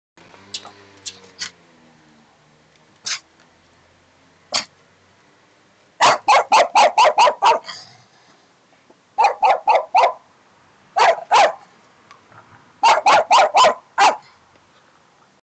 Funny barking dog in Hodmezovasarhely
Funny barking dog in Hodmezovasarhely. Video recorded with Samsung i8910 HD.